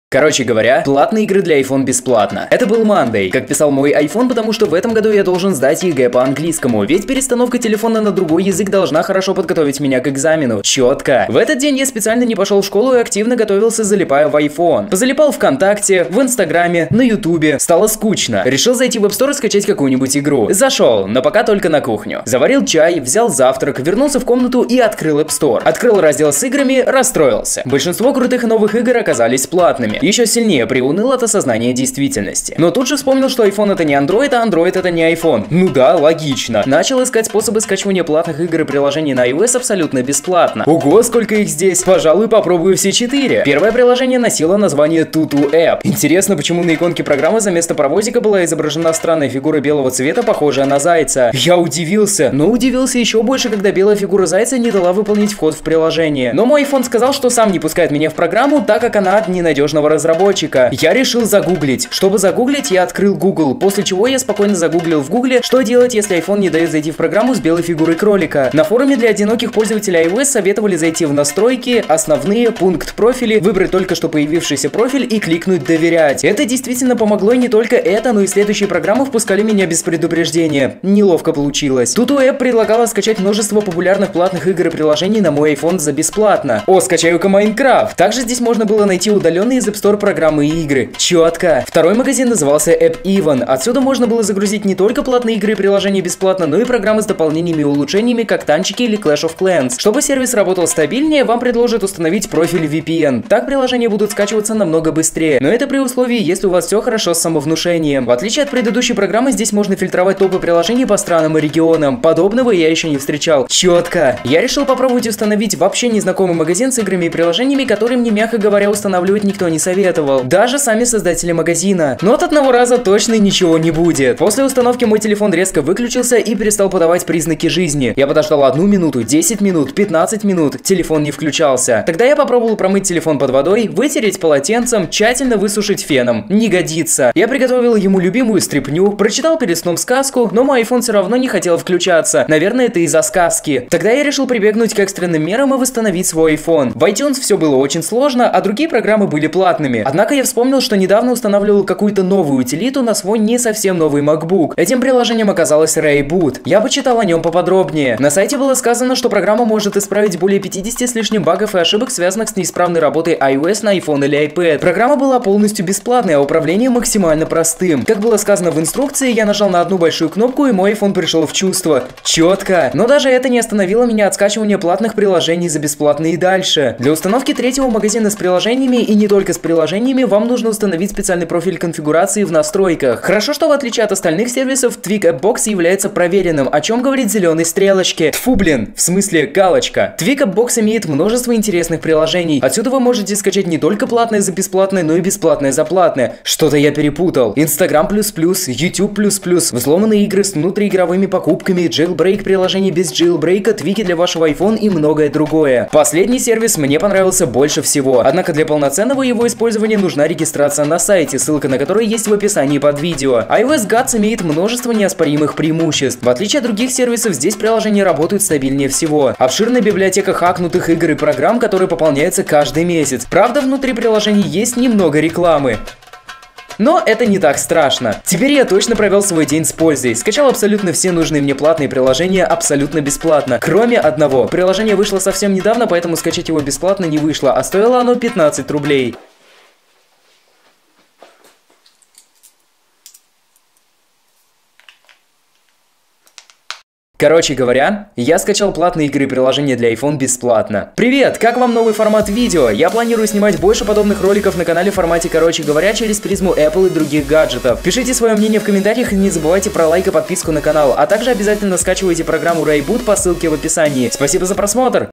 КОРОЧЕ ГОВОРЯ, ПЛАТНЫЕ ИГРЫ iPHONE БЕСПЛАТНО топ 10 игр на андроид
топ 10 игр на андроид .Решил остаться дома и позалипать в iPhone! Позалипал в VK, в instagram и на YouTube. Было скучно! Я решил залезть в App Store и скачать какую-нибудь новую игру
ПРИУНЫЛ! От осознания действительности, что большинство крутых игр и приложений оказались платными…
Поэтому я решил скачать платные игры для iPhone бесплатно! Нашёл 4 крутых способа с такими приложениями как TutuApp, AppEven, TweakAppBox и iOSGods
Все эти способы оказались реально крутыми! Поэтому я решил посоветовать их вам!

Друзья, это первое видео на канале в формате КОРОЧЕ ГОВОРЯ! Пожалуйста, оцените его в комментариях! Мне очень важно ваше мнение, так как я хочу снимать больше видео в таком формате через призму Apple, iOS, iPhone и технологий!